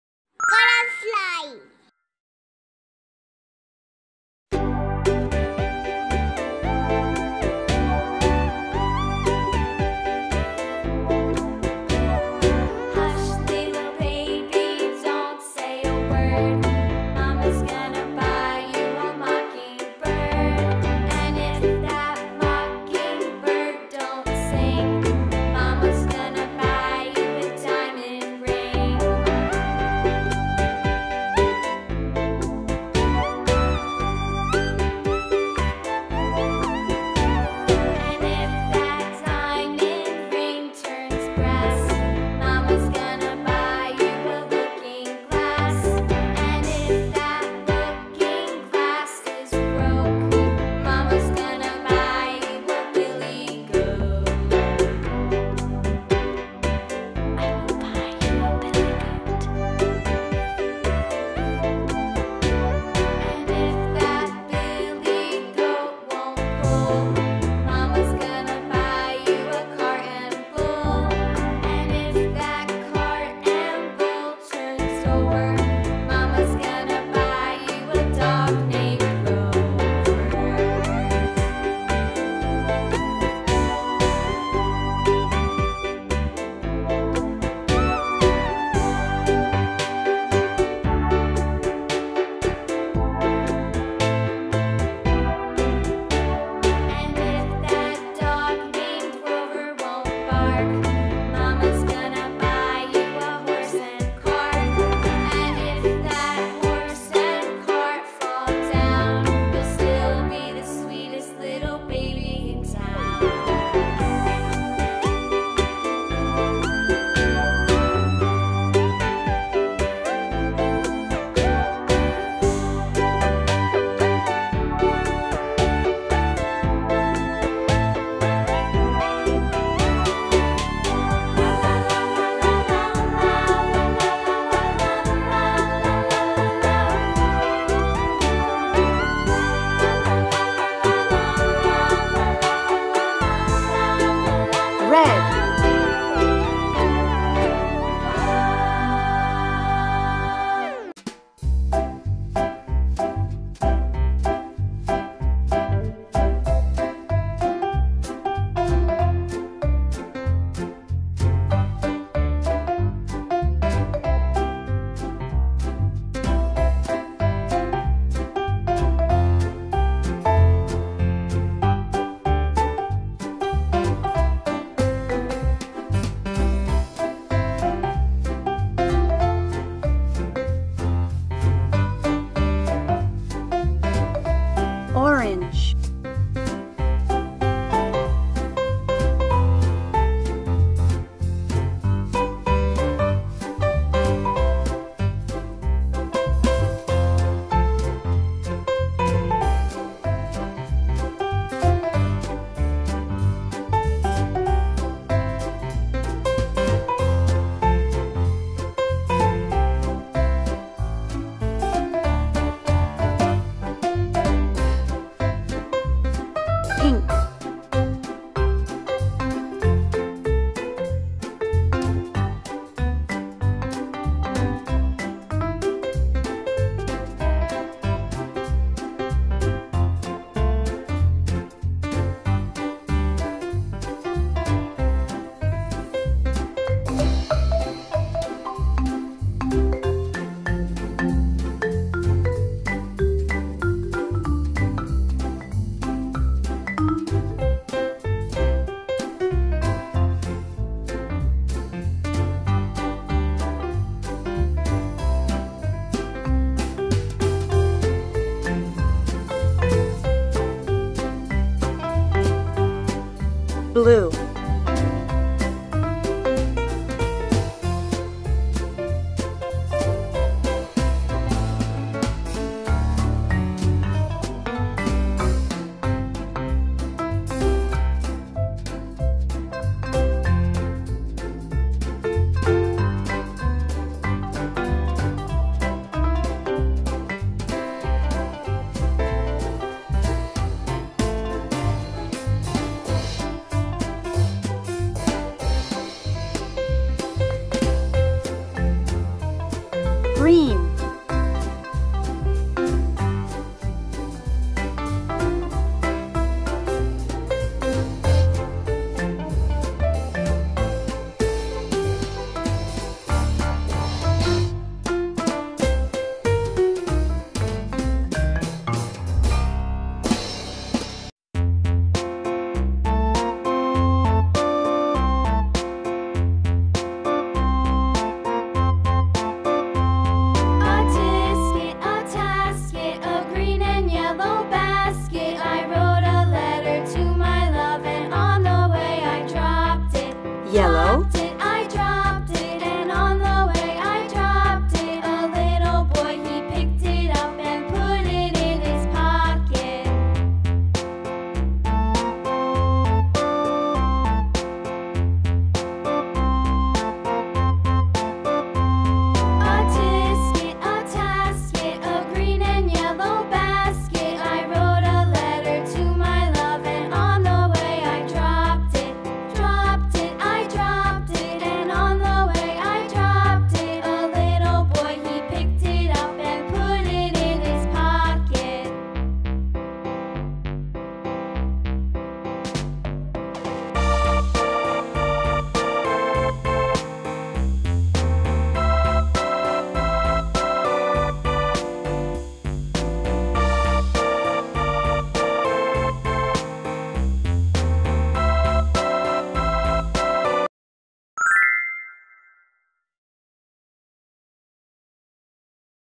How To Draw a Hat Step by Step For Kids and Toddlers
Hello Everyone!  Let's draw and paint a hat . Hope u like it. As it is a step by step tutorial for kids and toddlers you will feel easy to draw so..if u like it do like share and subscribe to my channel colourfly.